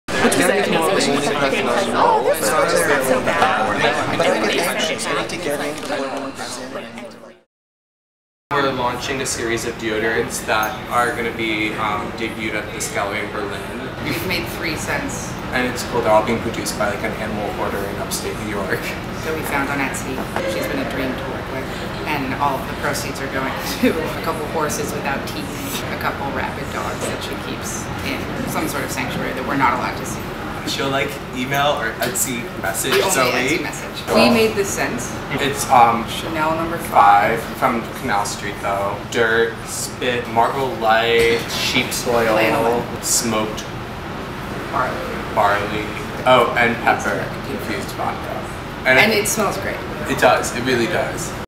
Eckhaus Latta Makes Scents | NYFW TMI
Eckhaus Latta branches out from post-futurist designs, to anti-futurist scents.